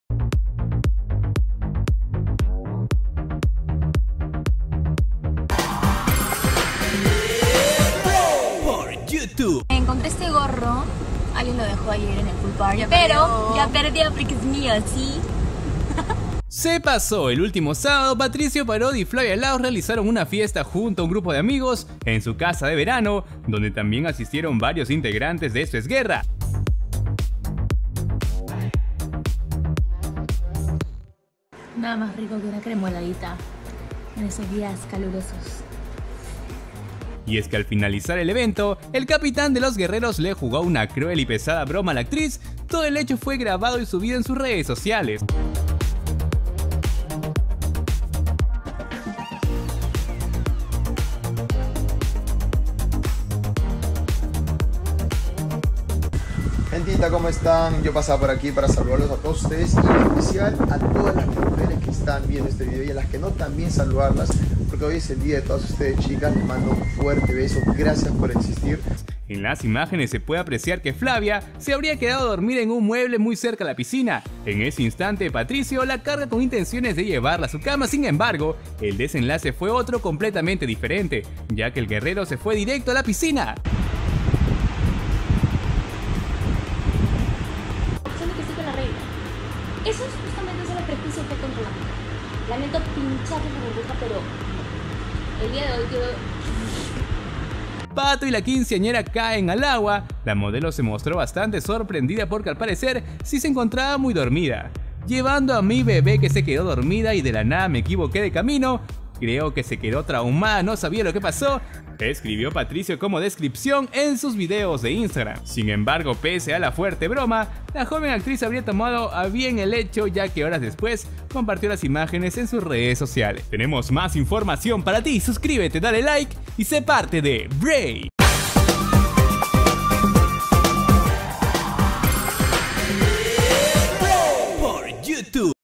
PATRICIO PARODI TIRÓ A LA PISCINA A FLAVIA LAOS MIENTRAS ELLA DORMÍA
¡Se pasó! El último sábado, Patricio Parodi y Flavia Laos realizaron una fiesta junto a un grupo de amigos en su casa de verano, donde también asistieron varios integrantes de “Esto es Guerra”.
Y es que al finalizar el evento, el capitán de ‘Los guerreros’ le jugó una cruel y pesada broma a la actriz.